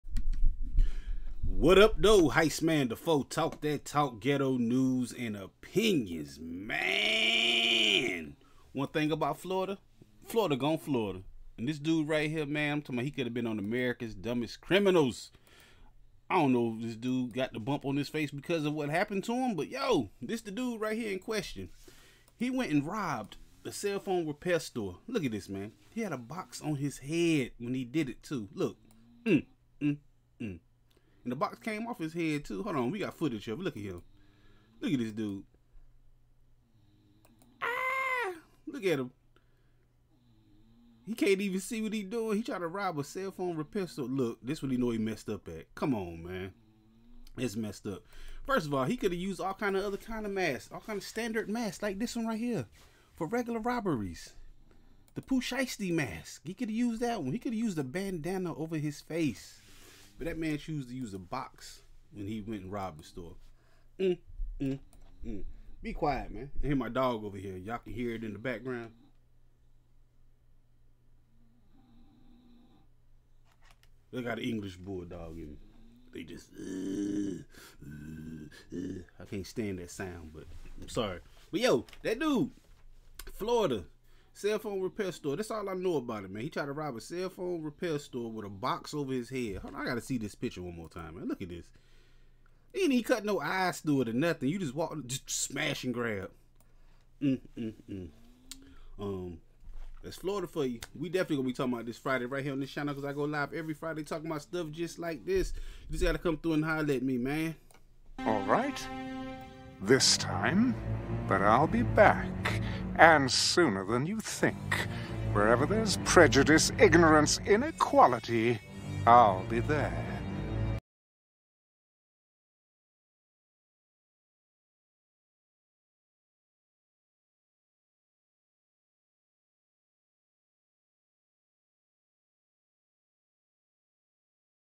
Man Wearing BOX On His Head Rob Phone Repair Store
Man in Florida uses a box to hide identity, while attempting to rob phone repair store.
Maaaaaaan!!!